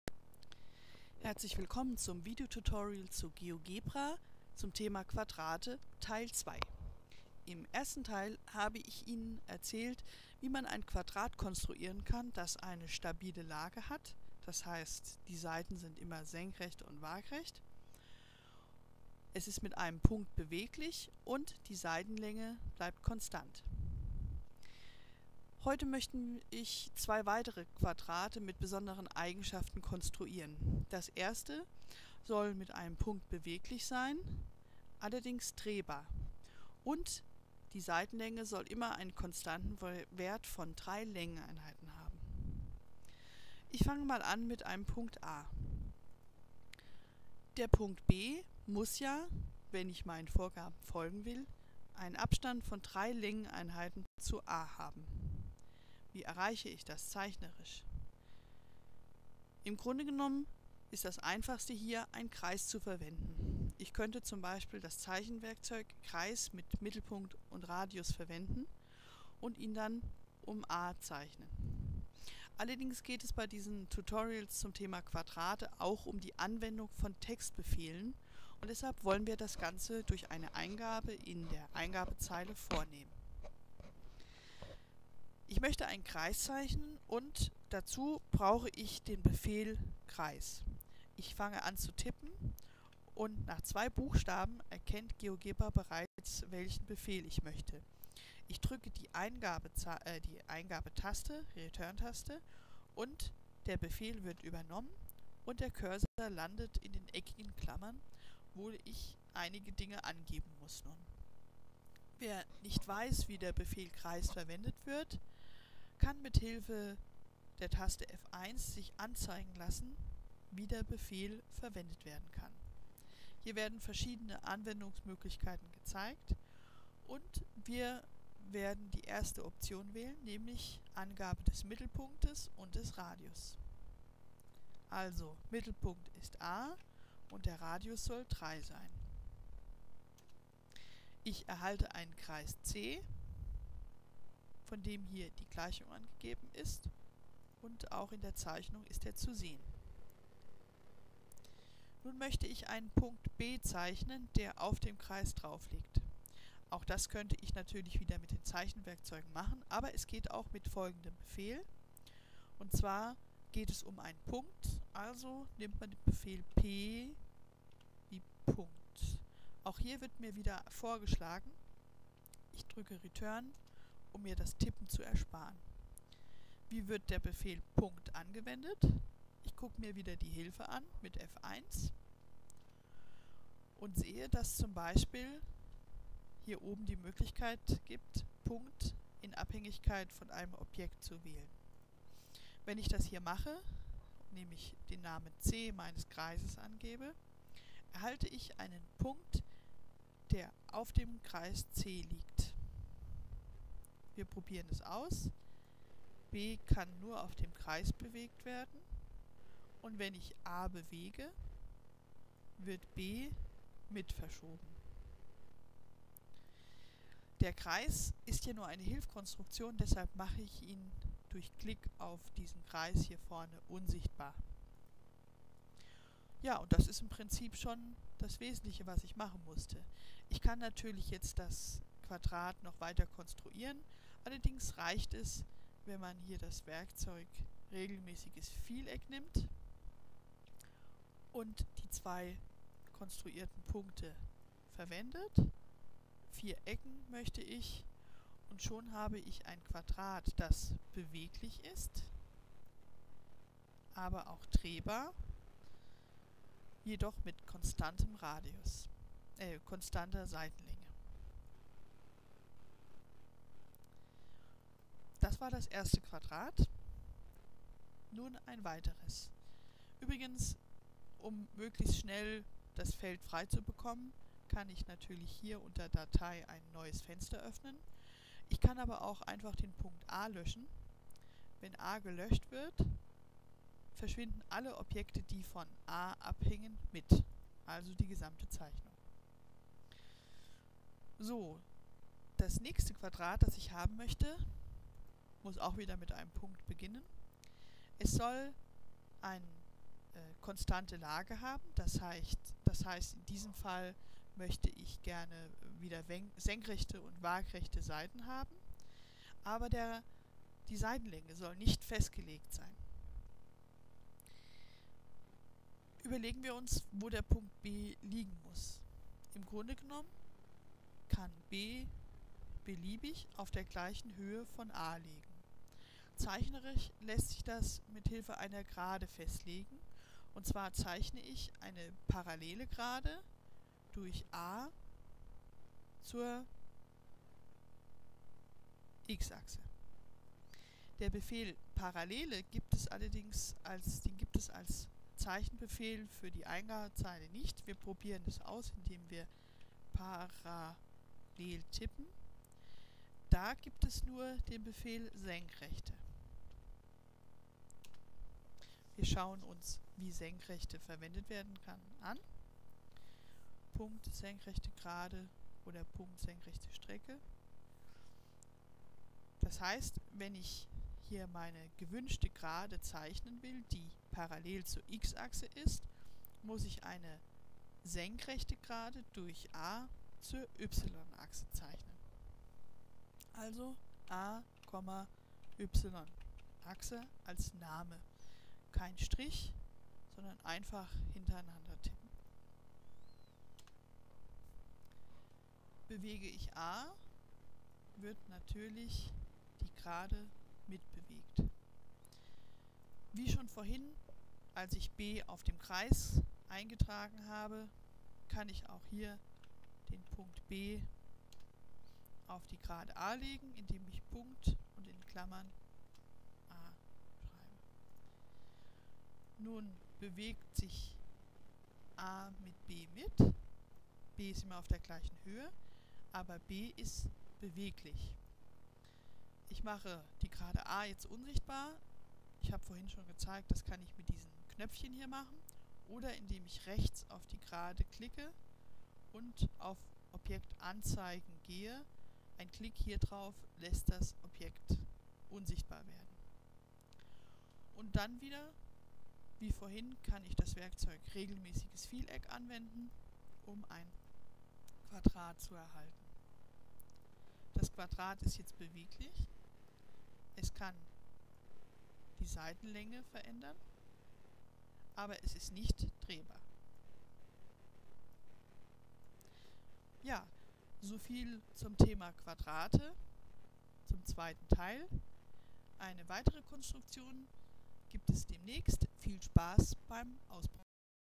Quadrate (2)
Ziel dieses und der weiteren Tutorials mit dem Titel "Quadrate" ist die "Konstruktion" verschiedener Quadrate mit speziellen Eigenschaften.
Allerdings werden hier nicht die Zeichenwerkzeuge genutzt sondern Textbefehle, die man in der Eingabezeile eintippt. Schauen Sie sich erst Teil (1) zu diesem Thema an.

Diese Tutorials ist geeignet, Geogebra-Anfängern zu zeigen, wie Textbefehle in GeoGebra genutzt werden können. Dazu gibt es wie immer hilfreiche Tipps.